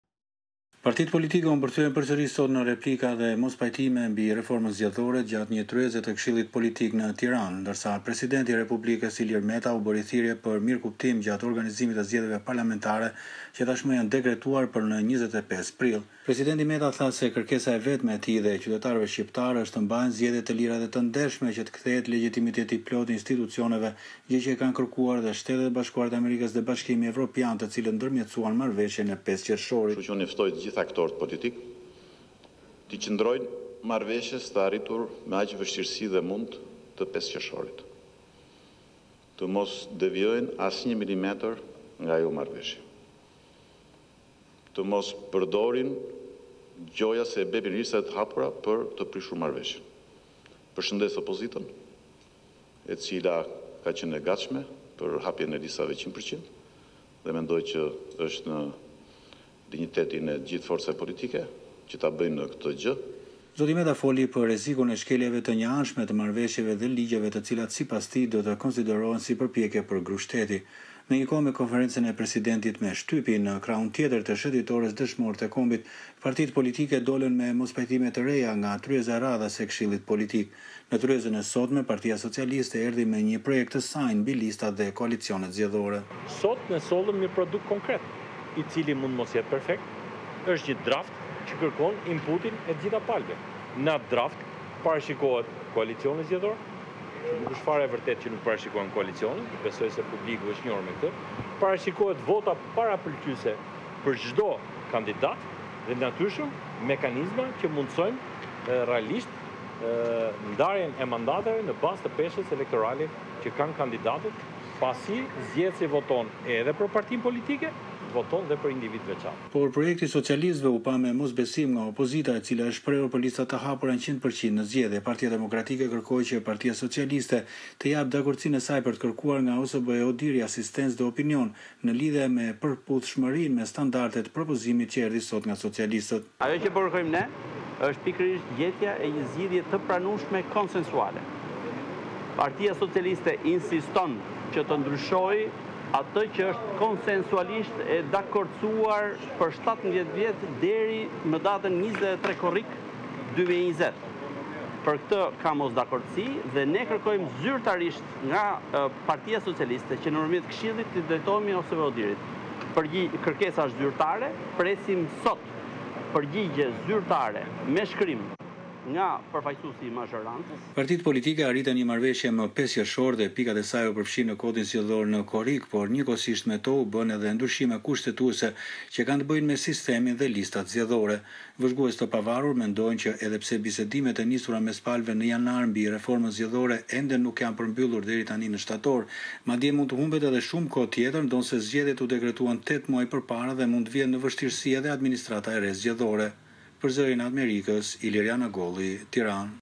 Tiranë: Vijojnë mospajtimet partiake mbi reformën zgjedhore
Sot (e premte) u mbajt një tjetër tryezë e Këshillit Politik, në përfundim të së cilës, u renditën sërish një seri mospajtimesh mbi çështje të procesit zgjedhor.